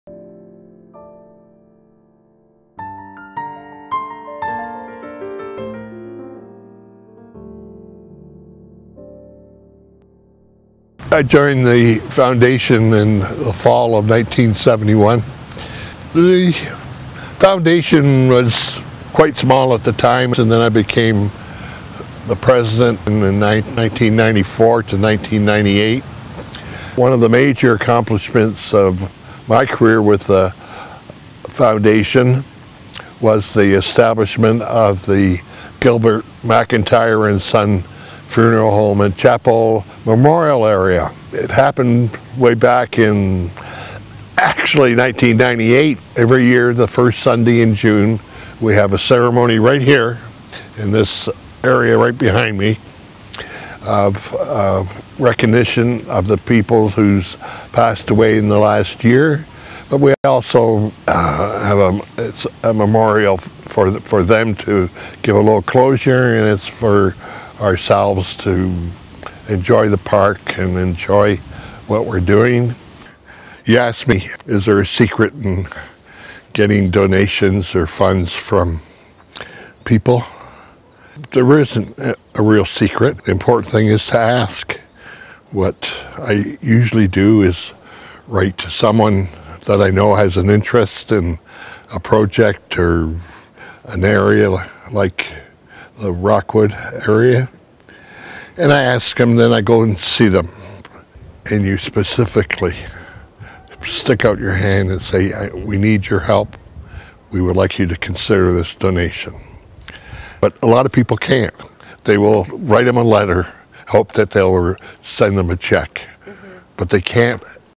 2009 Watershed Awards - Norm Lundvall
In 2009, Norm Lundvall received a Watershed Award for his commitment to fundraising projects with the Grand River Conservation Foundation.